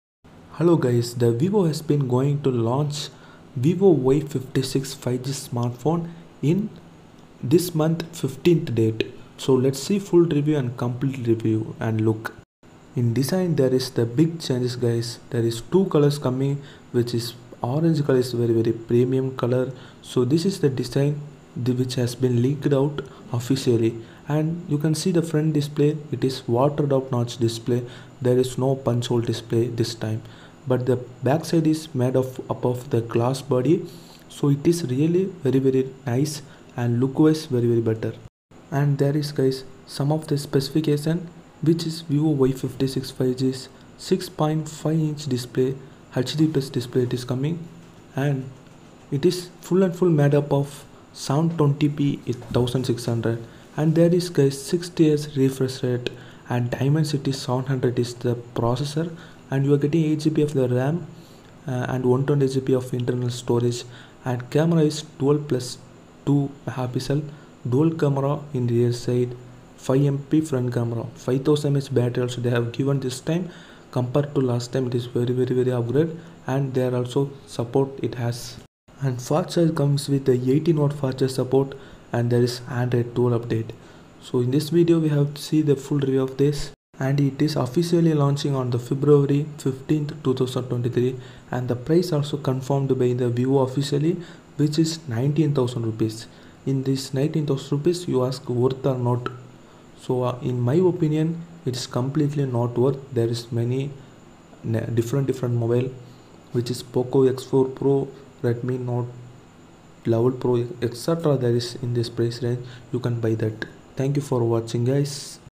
Vivo Y56 5G Review
Vivo Has Officially Going to Launch Vivo Y56 5G Smartphone In India

Vivo Y56 5G Price:₹18999/-
Vivo Y56 5G Specifications etc.. In this video!